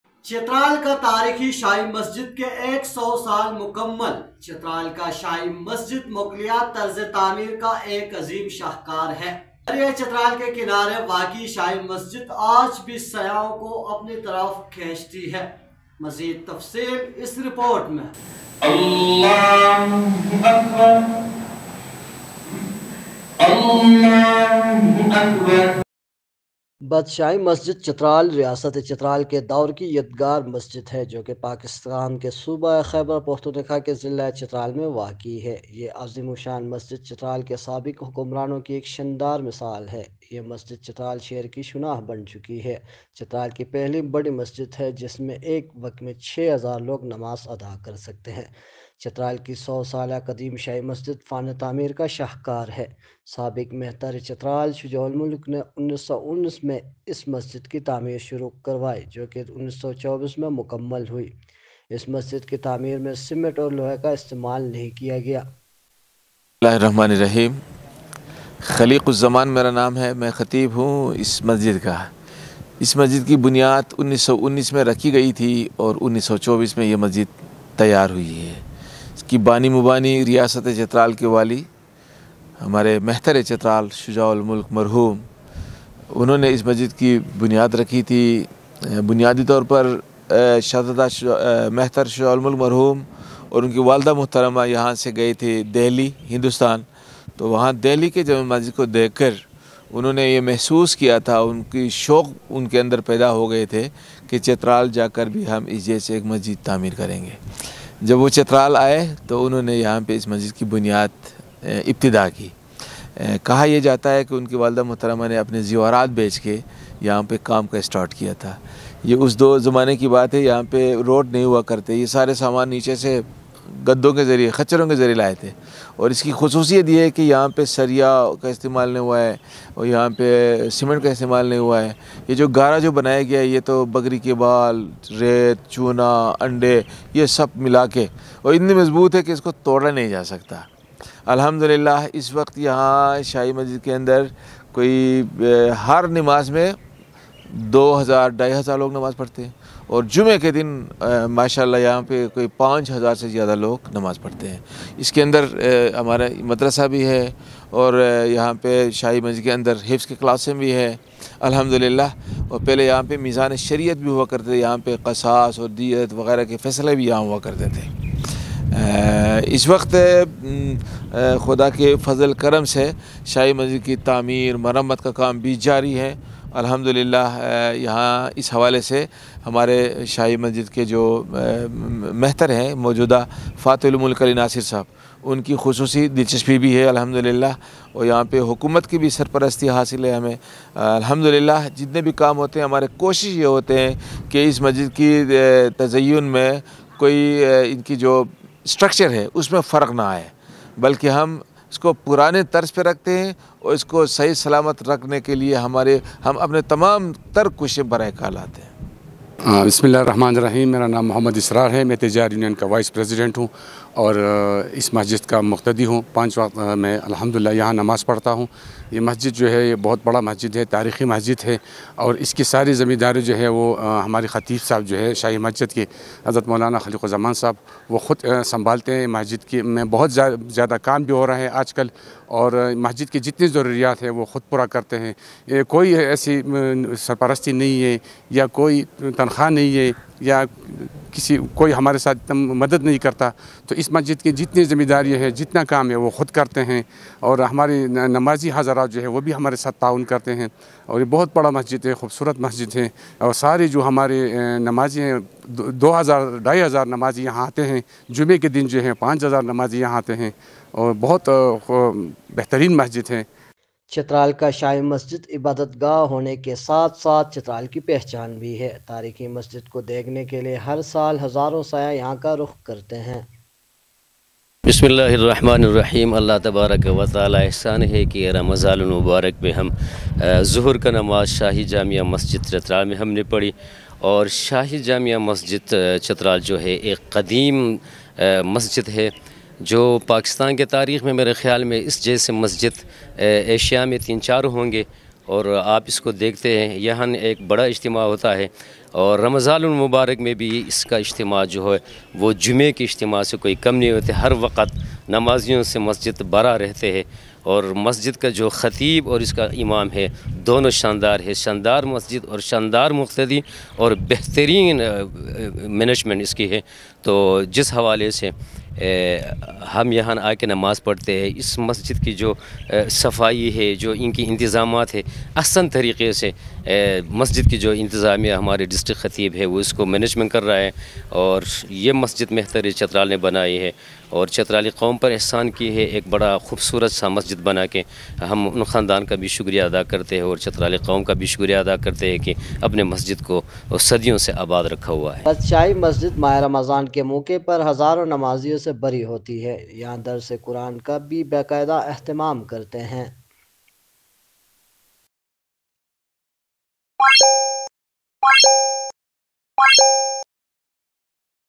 The historical mosque of Chitral has completed 100 years of Shahi Masjid | Ishpata News
The historical mosque of Chitral has completed 100 years of Shahi Masjid. The Shahi Masjid of Chitral is a masterpiece of Mughal architecture, the Shahi Masjid on the banks of river Chitral still attracts tourists.